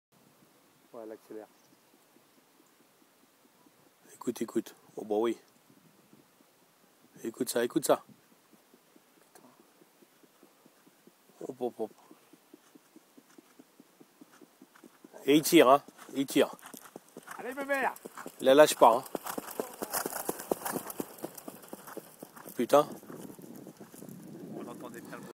UNA STORIA AIMEF ...PETIT BOULOT PREPARATOIRE AVANT PROCHAINE SA QUALIFICATION
EN DIGNE FILLE DE LE SEMI-CLASSIQUE PARNASSA AIMEF ELLE POSSEDE BEAUCOUP DE VITESSE. ELLE A UN MAGNIFIQUE POTENTIEL GENETIQUE POUR LA SUITE DE SA CARRIERE DE COURSES.